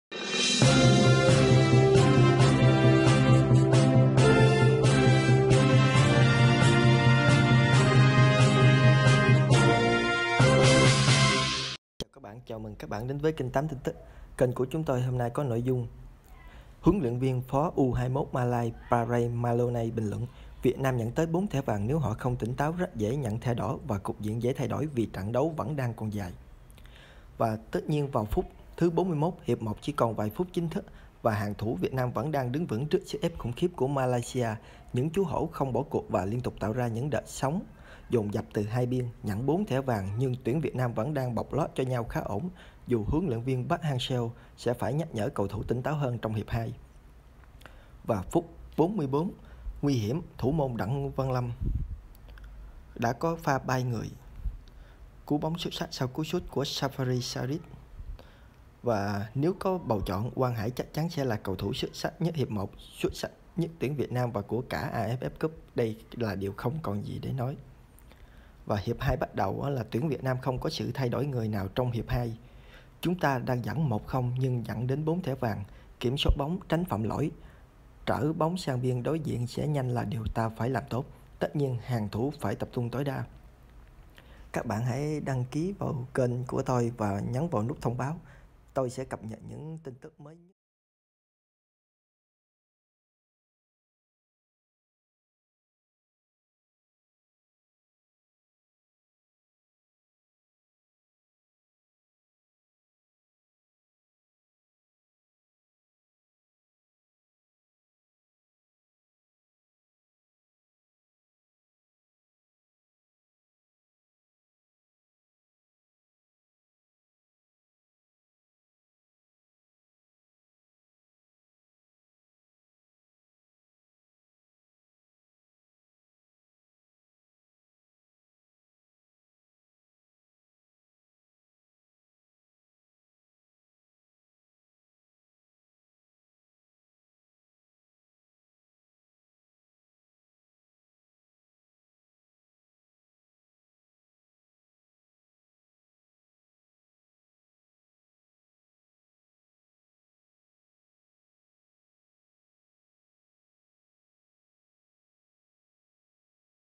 Việt Nam Thắng Chung kết lượt về với Malaysia, vươn lên vô địch
sân Mỹ Đình trước trận Việt Nam gặp Malaysia
Chiều 15/12, Đài truyền hình JTBC của Hàn Quốc có mặt bên ngoài sân Mỹ Đình từ rất sớm để ghi lại bầu không khí cuồng nhiệt trước thềm trận chung kết lượt về AFF Cup.